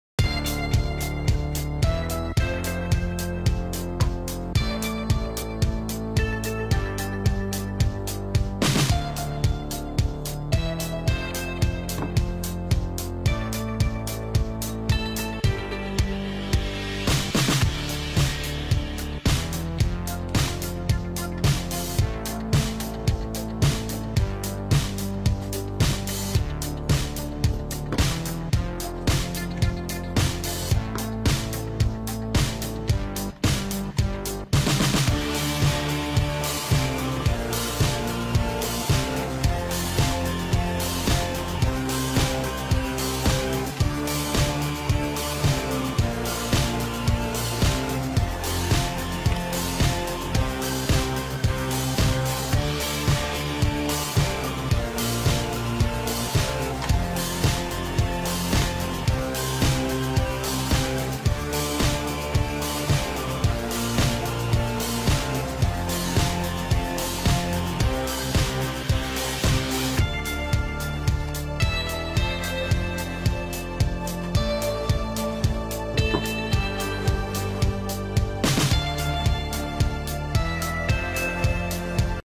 Шкатулка с секретом Ugears - деревянный конструктор, сборная модель, 3D пазл
Можете посмотреть как открывается шкатулка с секретом от Ugears. Собранная модель, готовая к использованию.